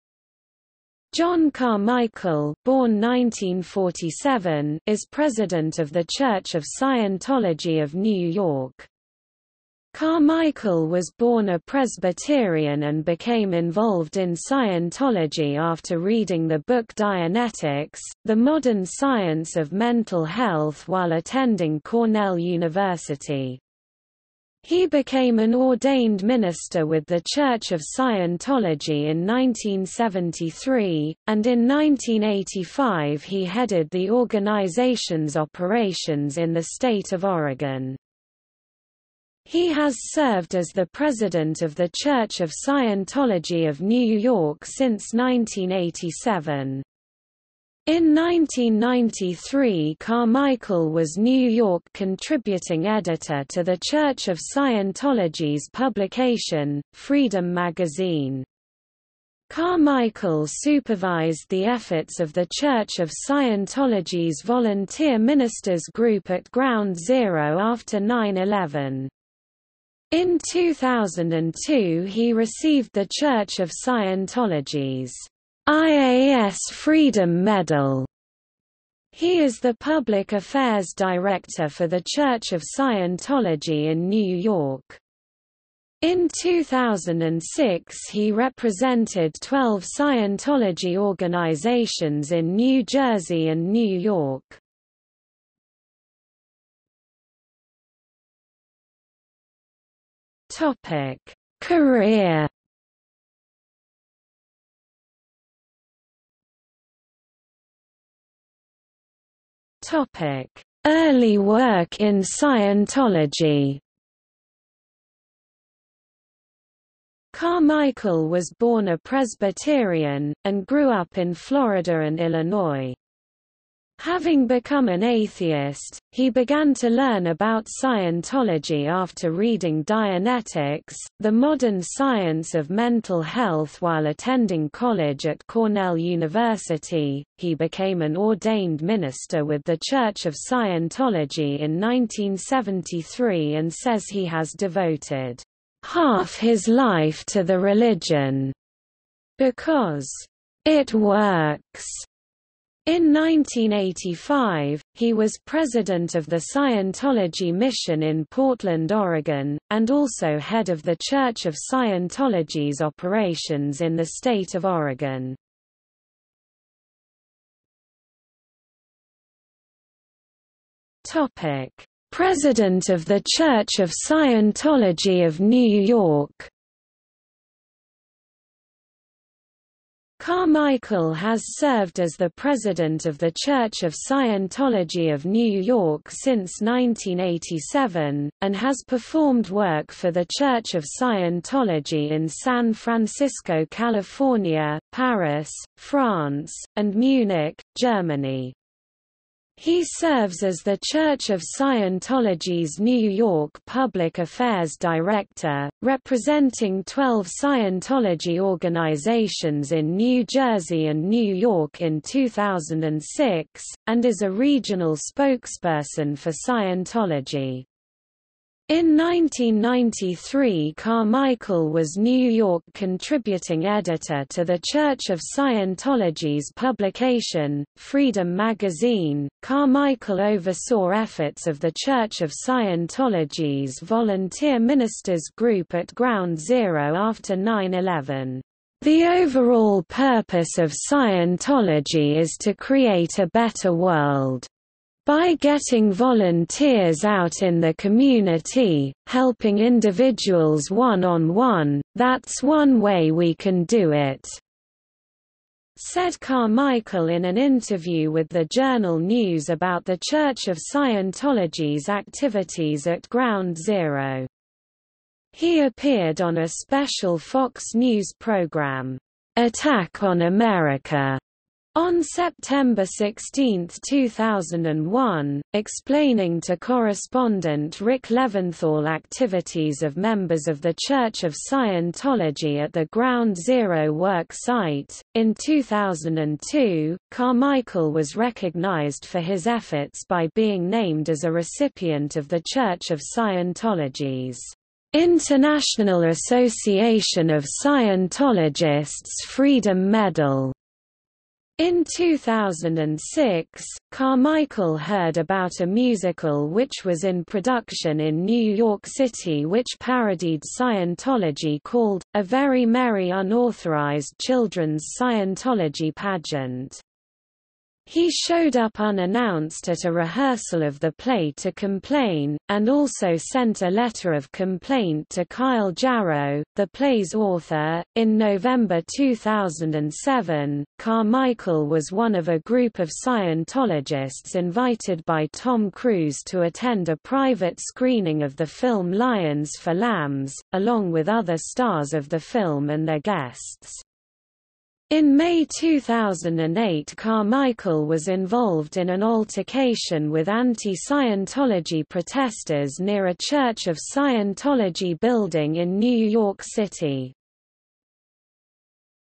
John Carmichael (Scientology) | Wikipedia audio article
00:01:36 1 Career
00:01:46 1.1 Early work in Scientology
00:02:47 1.2 President of the Church of Scientology of New York

- Socrates

SUMMARY
John Carmichael (born 1947) is president of the Church of Scientology of New York. Carmichael was born a Presbyterian and became involved in Scientology after reading the book Dianetics: The Modern Science of Mental Health while attending Cornell University. He became an ordained minister with the Church of Scientology in 1973, and in 1985 he headed the organization's operations in the State of Oregon.
He has served as the President of the Church of Scientology of New York since 1987. In 1993 Carmichael was New York Contributing Editor to the Church of Scientology's publication: Freedom Magazine. Carmichael supervised the efforts of the Church of Scientology's Volunteer Ministers group at Ground Zero after 9/11. In 2002 he received the Church of Scientology's "IAS Freedom Medal". He is the public affairs director for the Church of Scientology in New York. In 2006 he represented 12 Scientology organizations in New Jersey and New York.